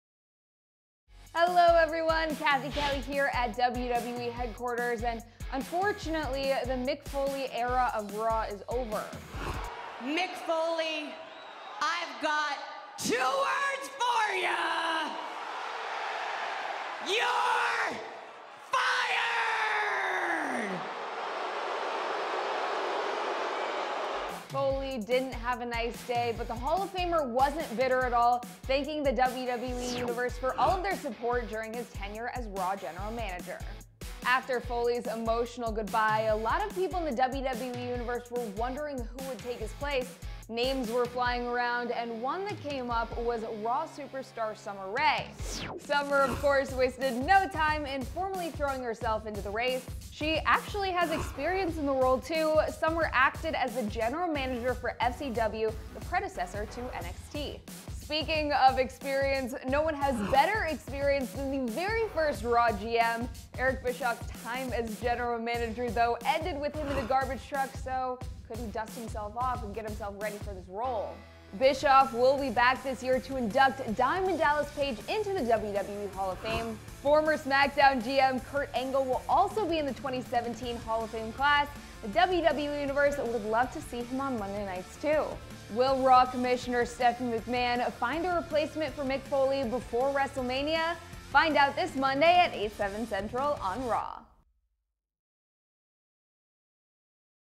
Summer Rae for Raw General Manager?
With Mick Foley no longer the General Manager of Raw, the WWE Universe has made a few suggestions on his successor.